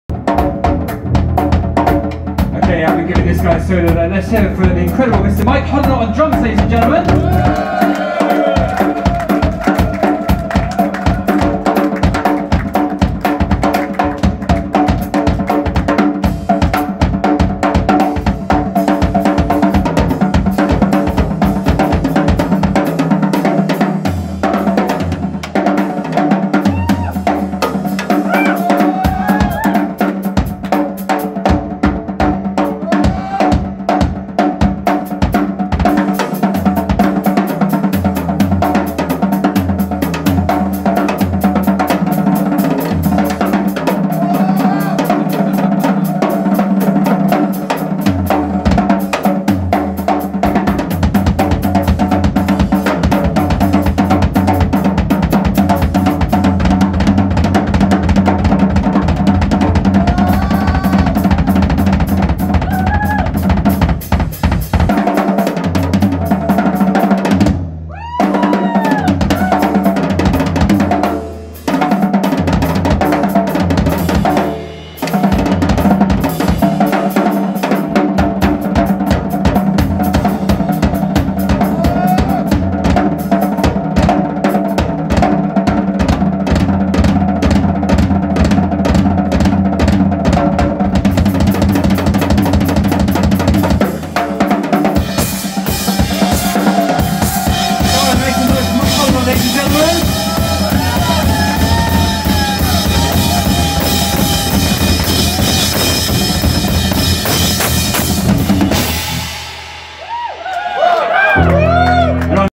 Drum Solo
Solo from the forth coming Ruzz Evans Live Album.